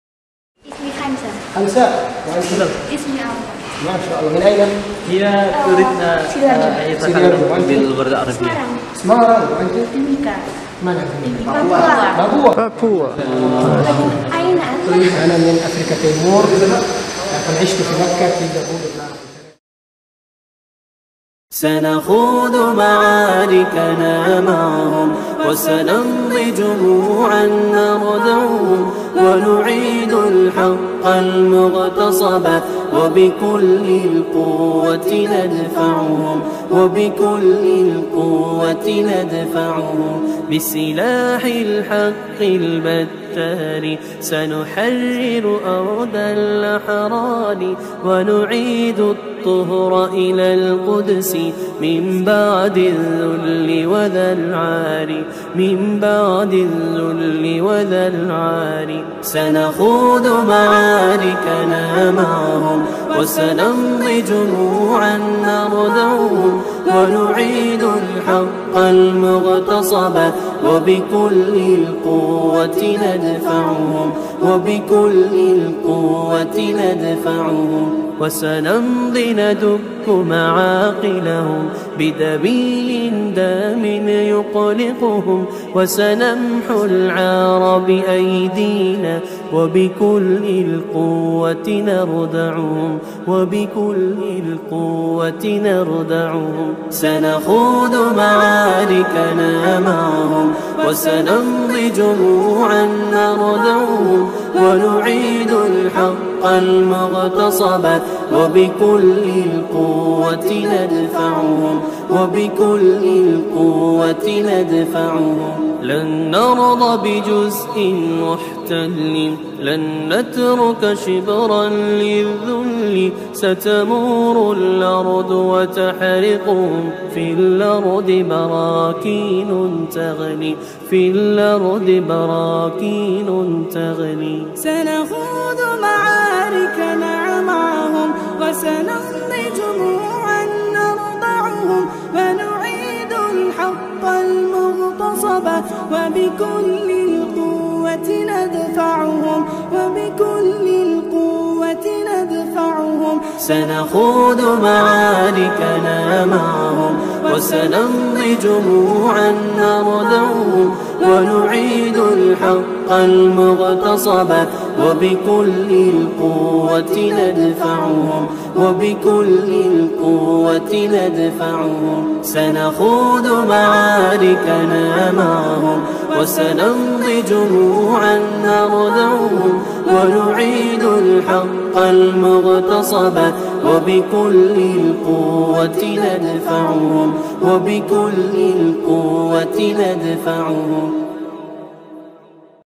Kunjungan Syaikh Utsman Shalih Ali (Pembina Kepesantrenan Al-Maahira IIBS Malang) 26 Oktober 2020
Syaikh Utsman Shalih Ali al-Makki Hafizhahullah adalah Pembina Kepesantrenan Al-Maahira IIBS Malang. Beliau berasal dari Eritrea, Afrika Timur. Lulusan Ma'had Darul Hadits Mekkah. Dan sudah lama tinggal di Indonesia.